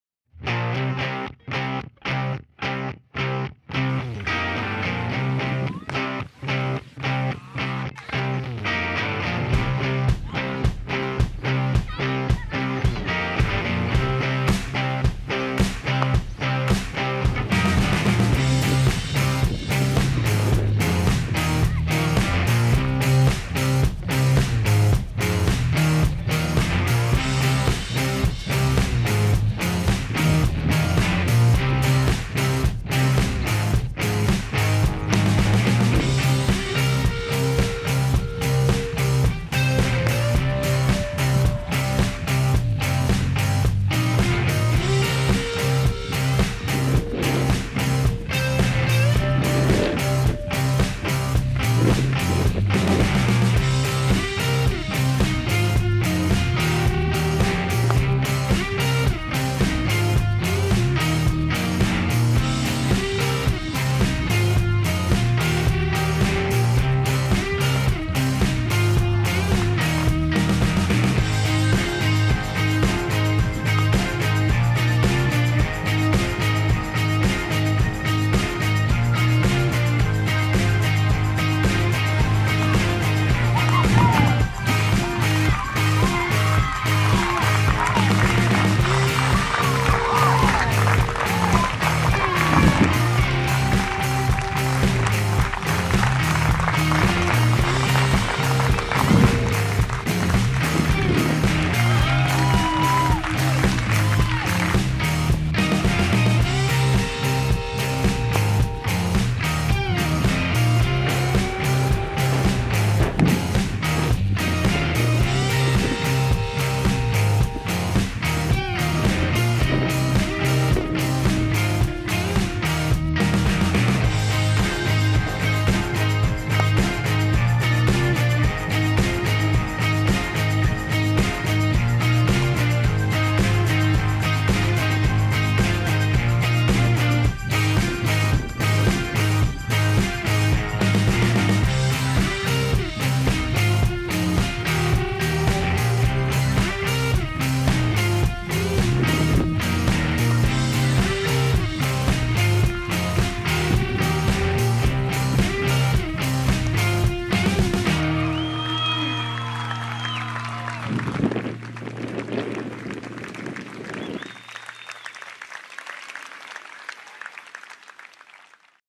Highlights of Rachel Priest's ODI 157
@WHITE_FERNS opener Rachel Priest hits a new career best and second ODI ton in the space of three ODIs against Sri Lanka in Lincoln, NZ, 7 November 2015